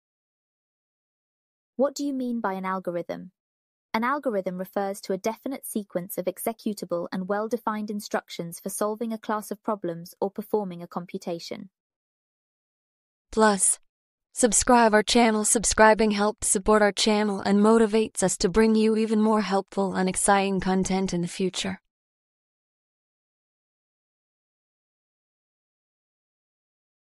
What do you mean by algorithm in computer?
An algorithm is a procedure used for solving a problem or performing a computation. Algorithms act as an exact list of instructions that conduct specified actions step by step in either hardware- or software-based routines.